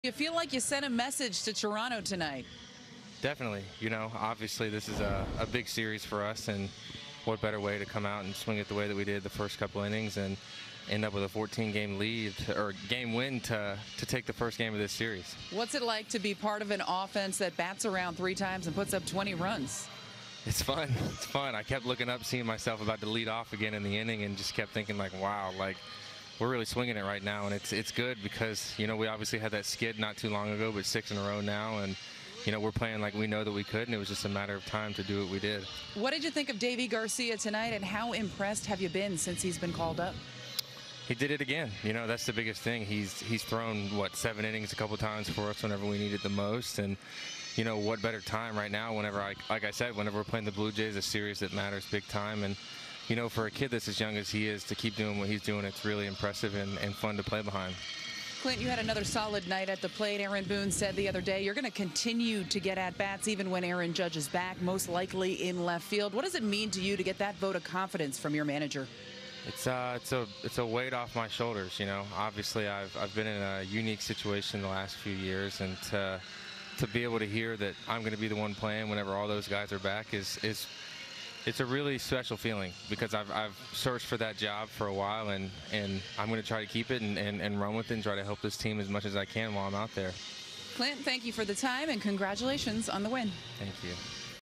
Clint Frazier homers, scores 4 runs in 20-6 rout vs. Jays
Clint Frazier was a major contributor in the Yankees' 20-6 blowout over Toronto in their series opener Tuesday night in the Bronx.